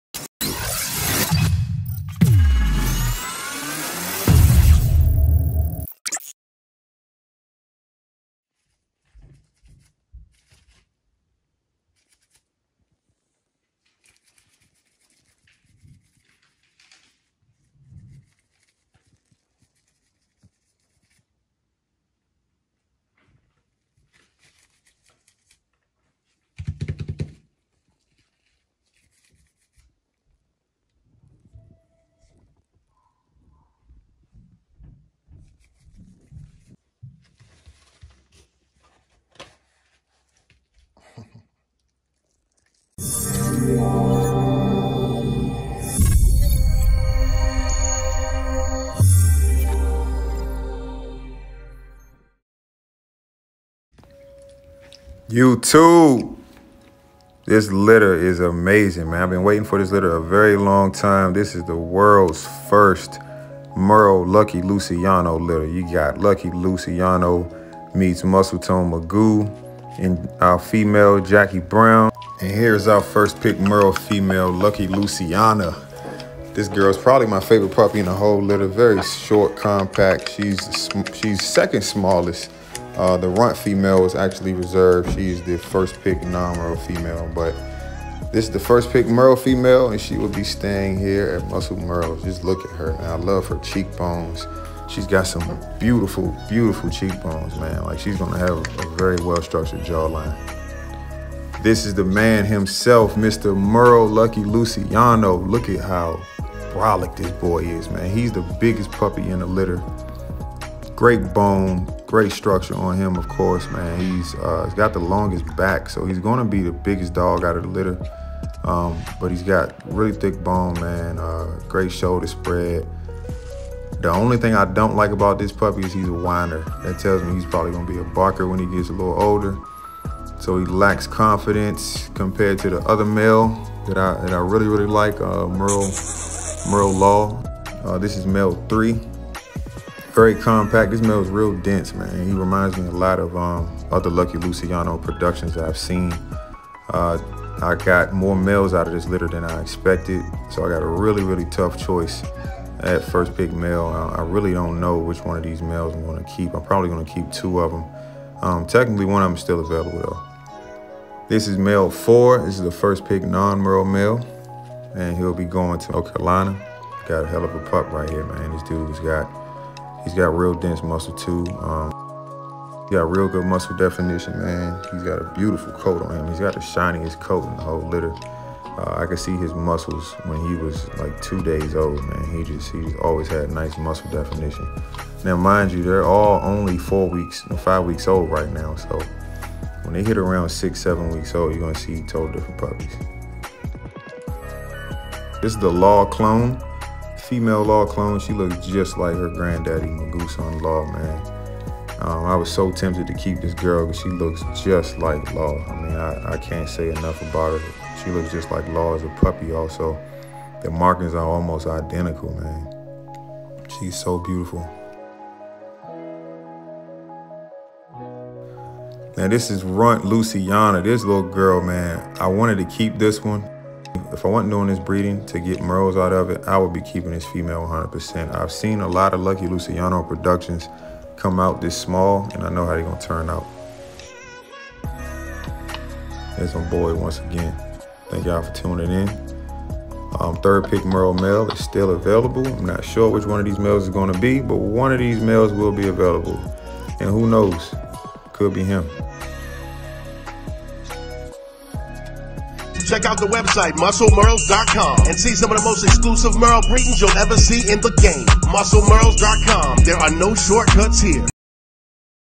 AMERICAN BULLY Puppies: World’s First Merle Lucky Luciano Litter at 4wks MuscleMerles.com
5 week old American Bully Puppy updates

Worlds first Merle Lucky Luciano Litter
Lucky Lu X Muscletone Magoo

InShot music credits
Musician: @iksonmusic